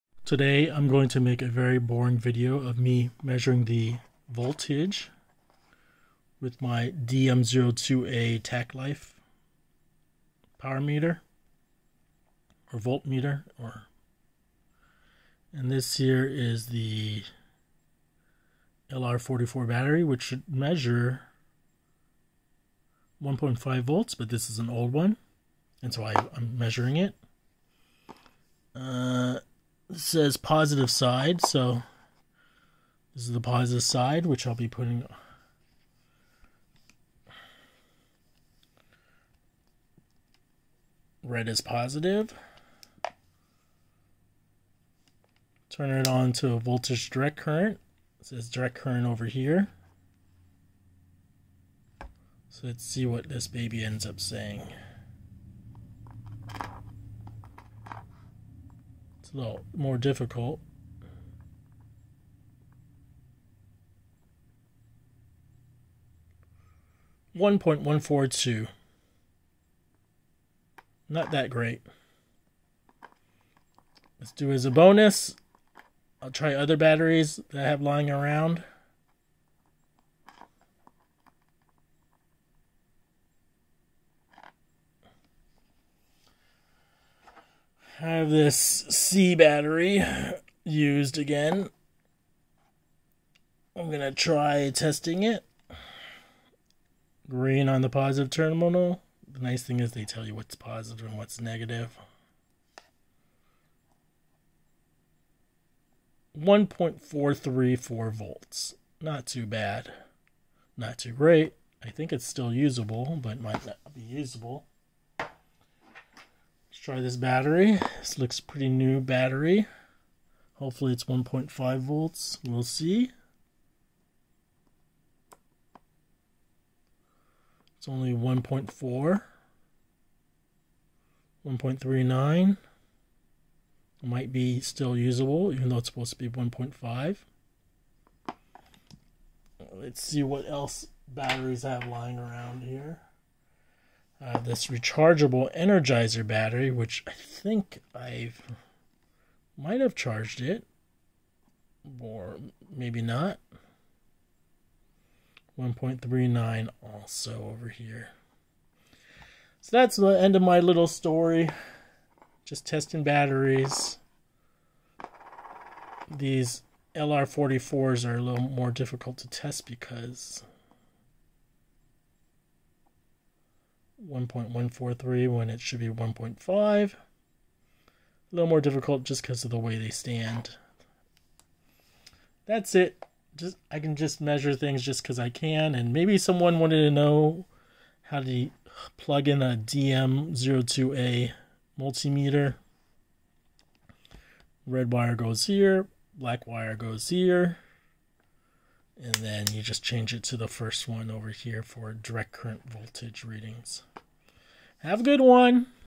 check battery voltages on some old/small lr44 batteries. Using Tacklife DM02A voltage multimeter
Title says it all.. nothing interesting.

LR44 batteries are supposed to be 1.5V

These batteries were dead I think.